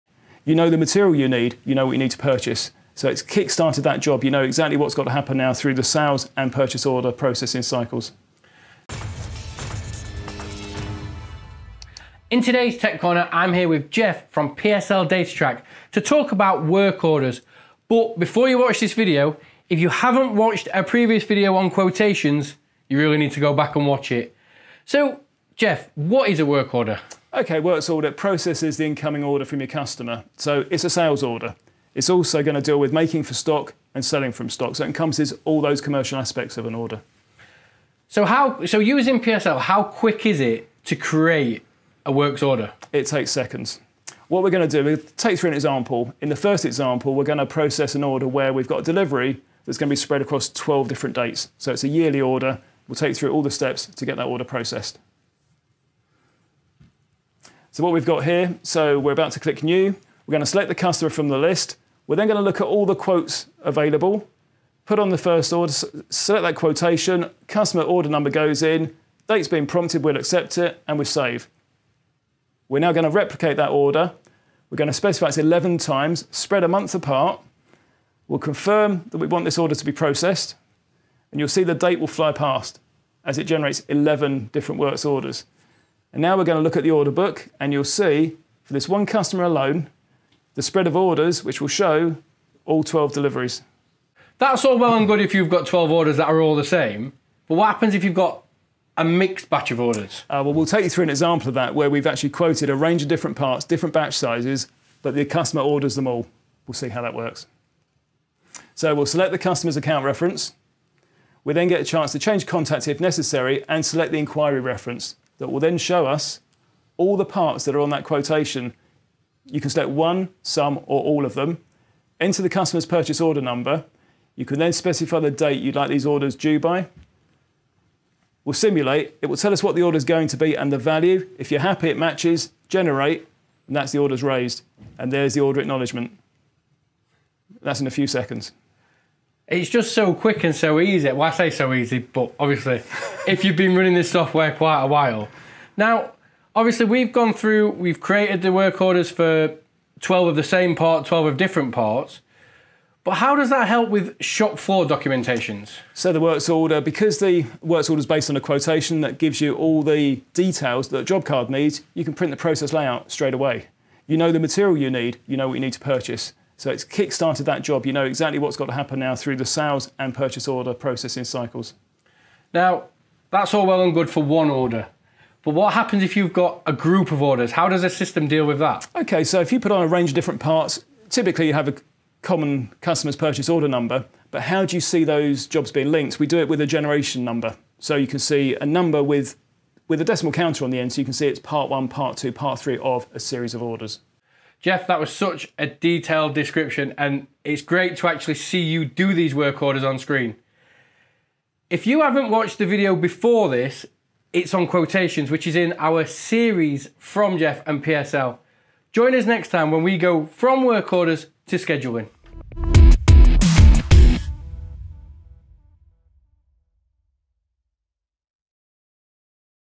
Automated works order processing with PSL Datatrack
In the manufacturing world, time is money and with the ability to create a works order in seconds, PSL Datatrack allows you to get more done quickly and efficiently! Join Tom Skubala of MTDCNC and Geoff Gartland of PSL Datatrack for an exciting demonstration of the flexible software that lets you quickly generate and replicate multiple works orders. PSL Datatrack can help you easily kickstart jobs, giving you more control over your sales and purchase order processing cycles.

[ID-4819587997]